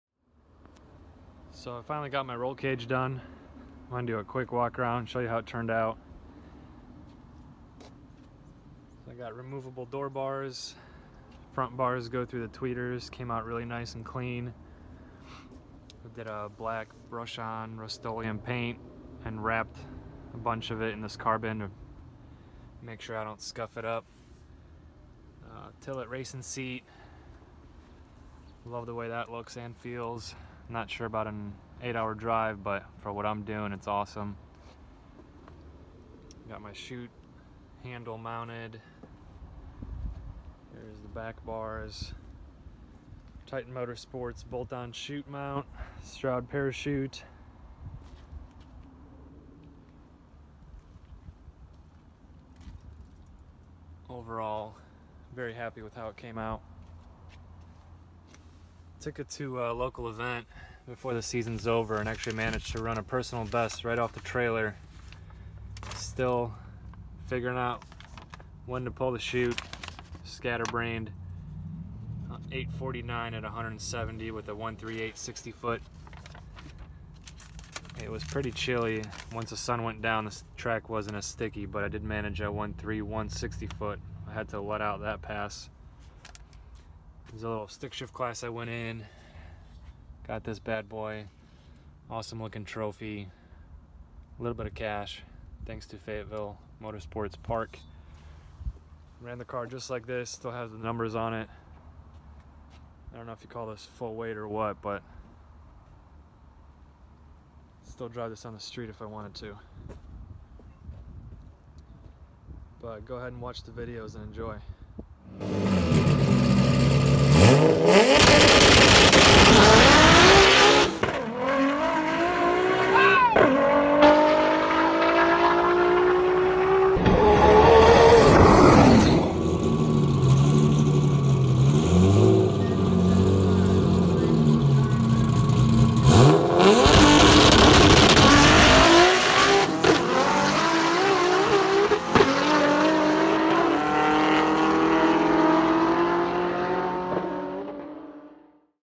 Part 9: Cage done & 8.49@170mph
Finished up the 10 point cage, Tillett B5 seat, and chute just in time to make one of the last local events of the year.  Still getting used to everything I managed a personal best of 8.49@170 right off the trailer.  As a reminder 3.0 liter, OEM 6 speed trans and a Garrett GTX4508r.